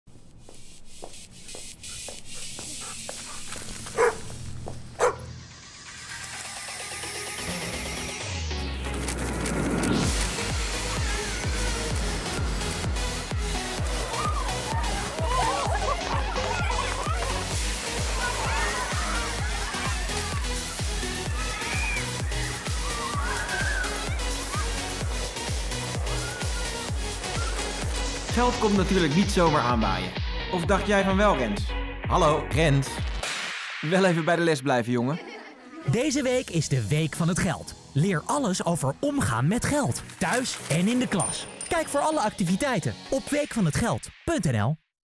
Televisie commercial Week van het geld
Doe mee met de Week van het geld en leer van alles over omgaan met geld.

Van maandag 27 t/m vrijdag 31 maart 2023 vieren we de 12e Week van het geld.